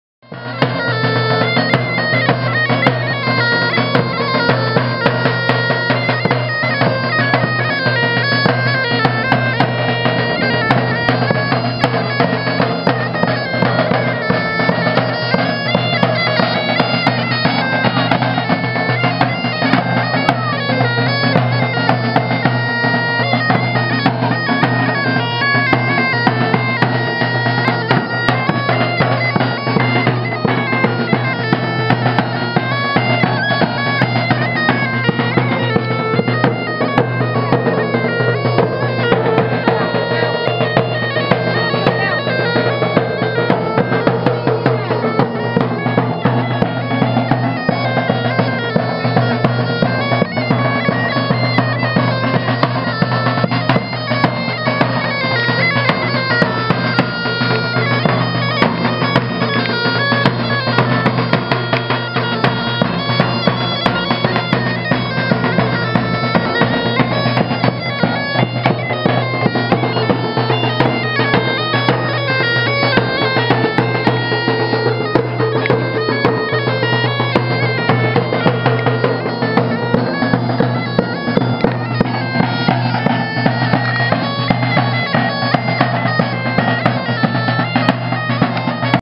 Cortejo Celta, Solstício do Verão, Chãs – Grupo Lua Nova Gaiteiros de Mogadouro
Lua Nova-Gaiteiro de Mogadouro – Numa breve pausa do cortejo Celta, a caminho dos Templos do Sol, Maciço dos Tambores, arredores da aldeia de Chãs – Foz Côa – para a celebração do Solstício do Verão, dia 21 de Junho, 2015 - Num local onde em tempos pré-históricos os homens louvavam o Deus Sol e ofereciam os seus sacrifícios – – Mais pormenores